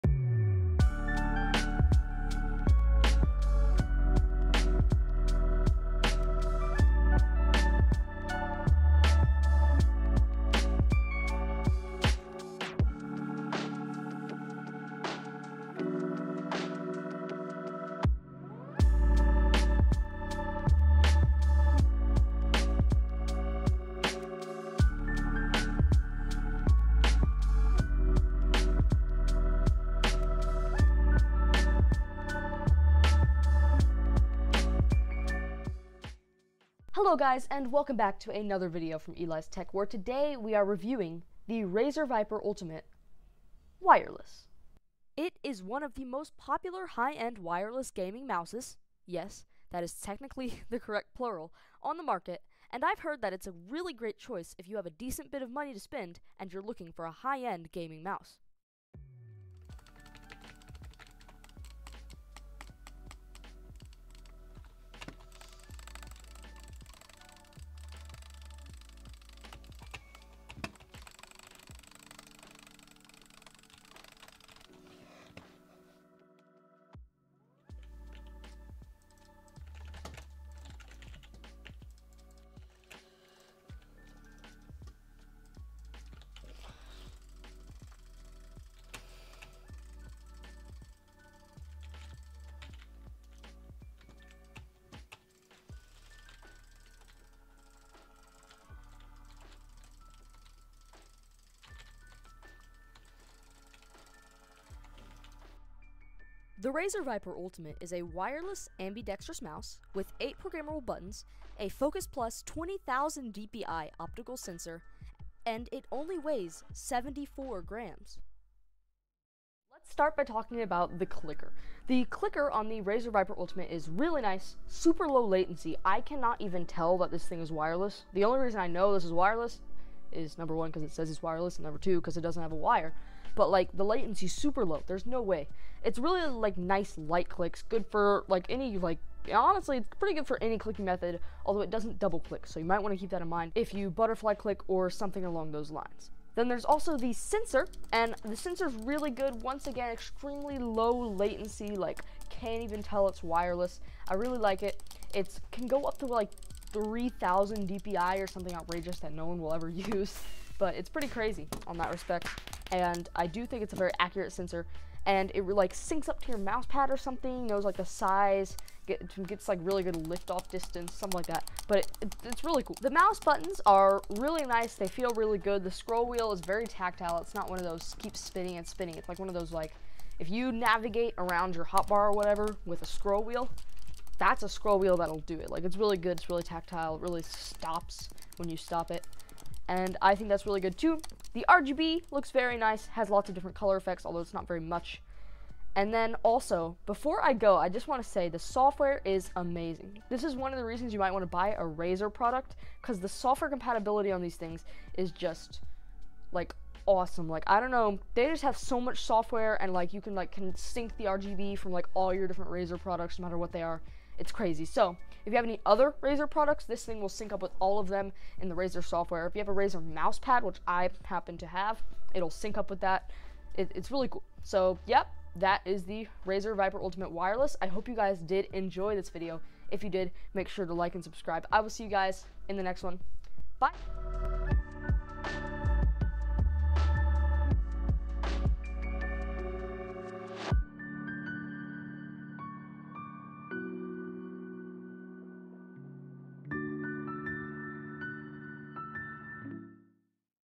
The $100 Gaming Mouse!
In this video, I show you guys a $100 Gaming Mouse! This mouse costs $100, and it's great for gaming!

My PC specs:
CPU: Ryzen 5 3600 6 Core Processor
GPU: XFX Radeon RX 580 GTS XXX
RAM: Patriot Viper Steel 2x4 DDR4-3200